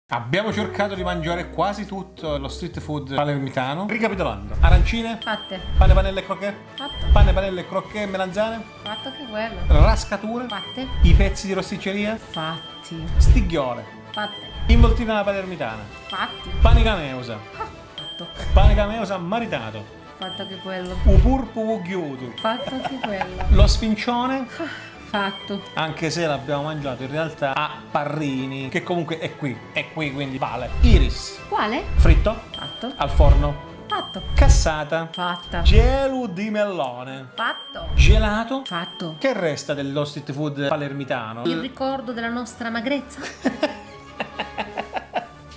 TUTTO (o quasi) lo STREET FOOD di PALERMO!!!
Siamo stati a Palermo ed abbiamo assaggiato quasi tutto lo street food della città!
Cosa manca? Scrivetecelo nei commenti!

Buon divertimento, buon viaggio e buon appetito!

Alessia e Fabrizio